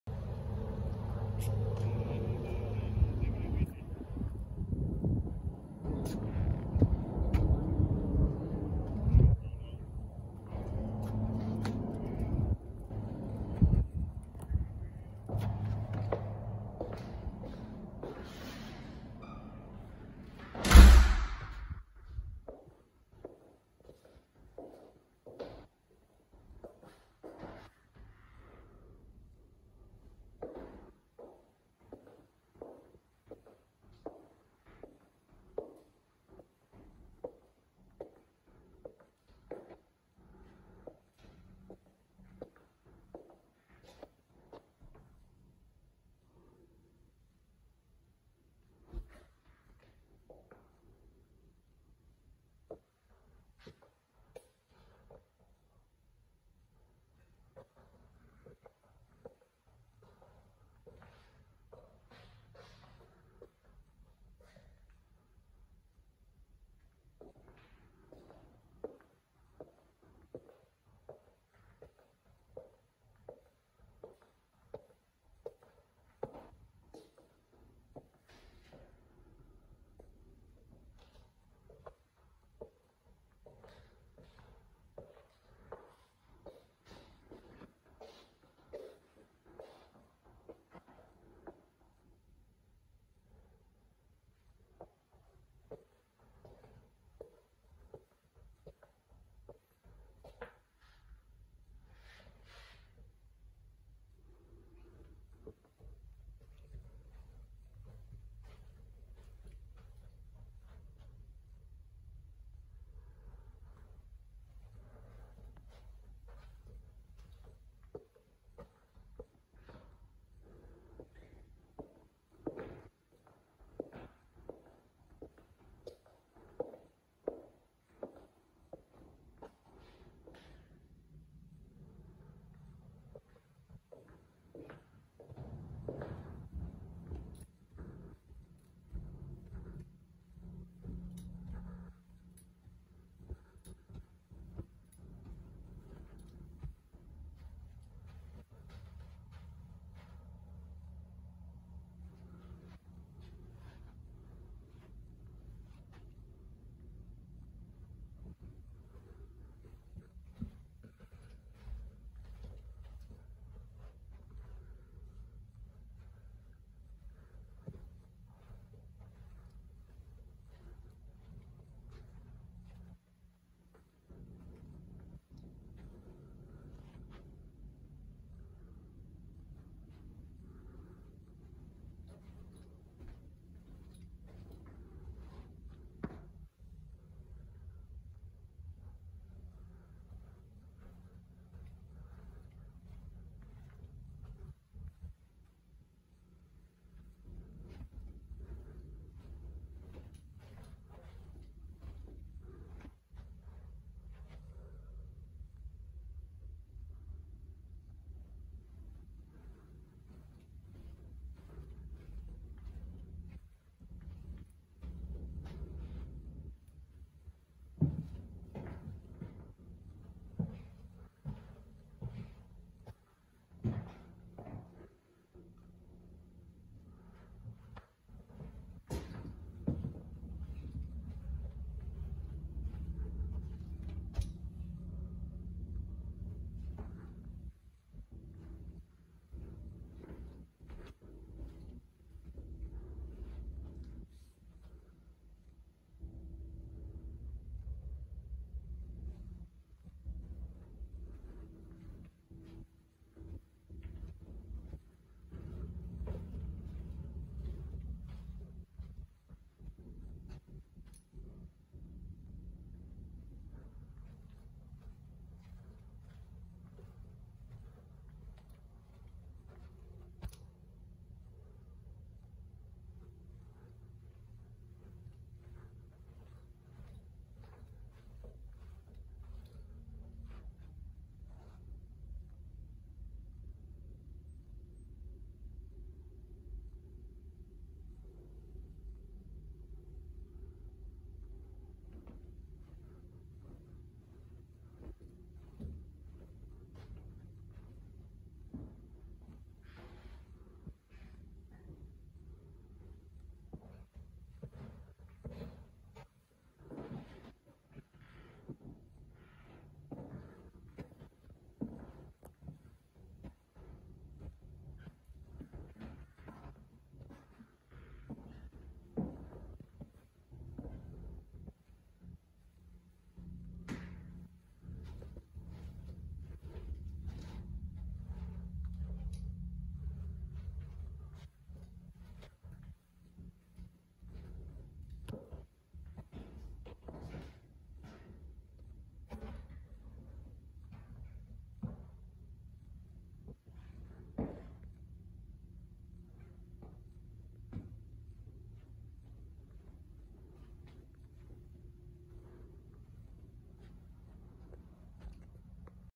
The Hayden plan 5 br 3 bth
This is a nice spacious home with guest bedroom on main and loft upstairs.